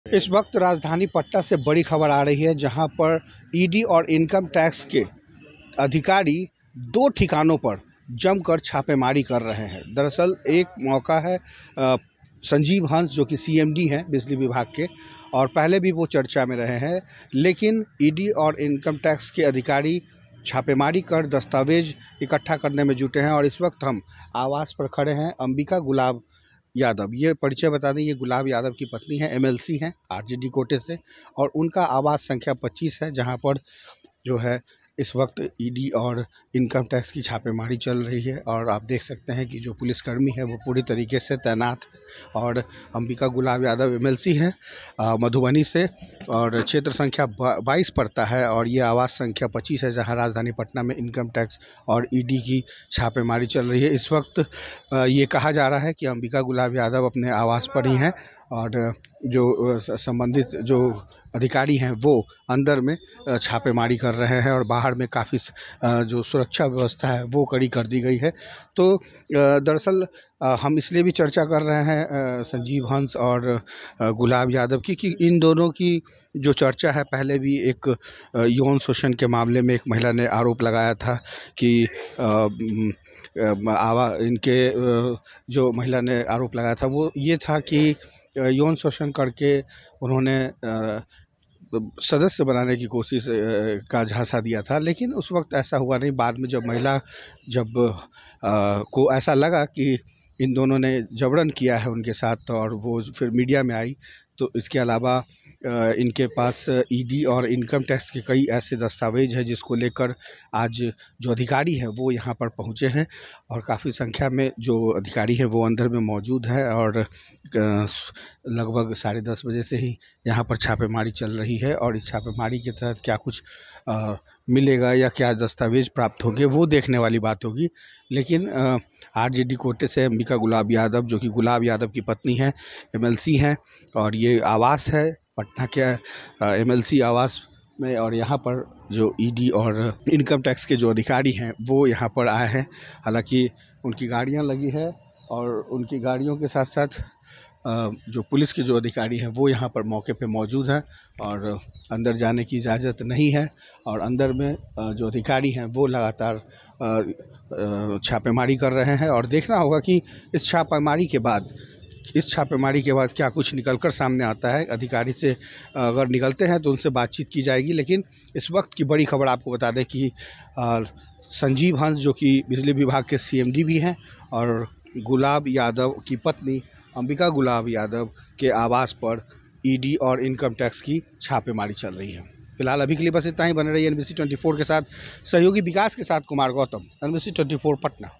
बिग ब्रेकिंग गुलाब यादव की पत्नी अंबिका गुलाब यादव के आवास पर देखिए कैसे चल रही है ED और इनकम ..?
बिग ब्रेकिंग गुलाब यादव की पत्नी अंबिका गुलाब यादव के आवास पर देखिए कैसे चल रही है ED और इनकम टैक्स की छापेमारी !